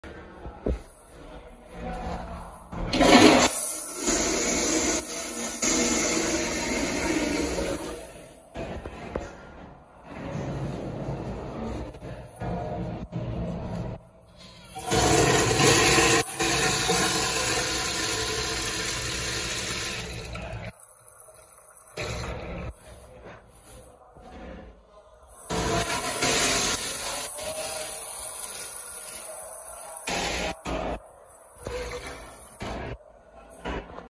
Epic fail…
No seat, no stall door, and absolutely crap fixtures. This was at 125 St on the A, B, C and D lines. 1/10